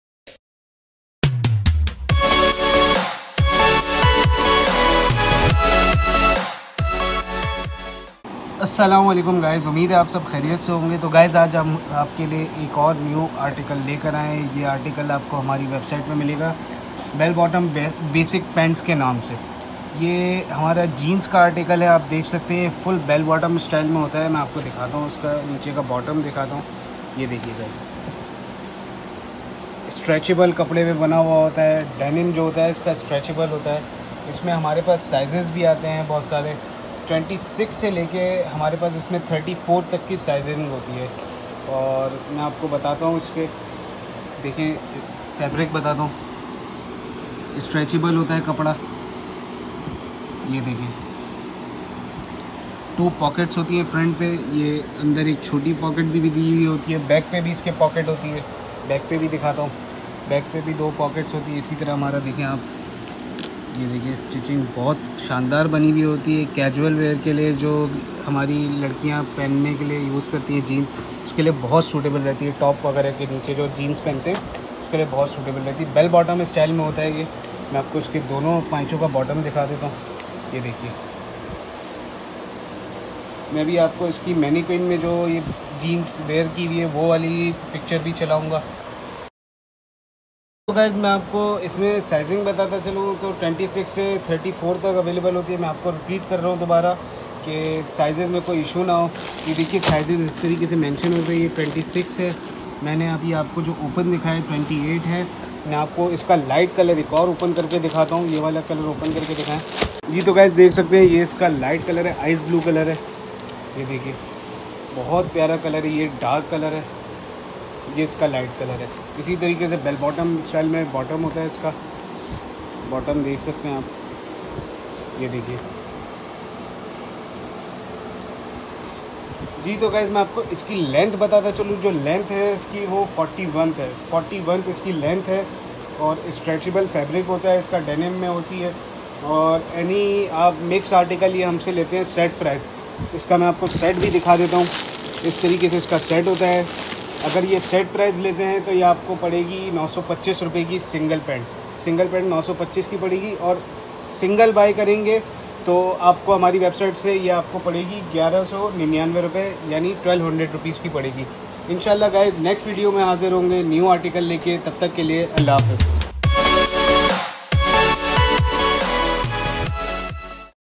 LOLLYPOP PRET OFFICIAL || LP BELL BOTTOM BASIC || DENIM JEANS || KUNAR CLOTHING
🖲Live product review 📹
LP-BELL BOTTOM BASIC
🔖Price 1199/-
▪️Fabric DENIM (stretchable)

▪️Waist availables  26 to 34
▪️Length 41 (boot cut)

Reseller 999/-
Bulk 925/- set price
made in Pakistan 🇵🇰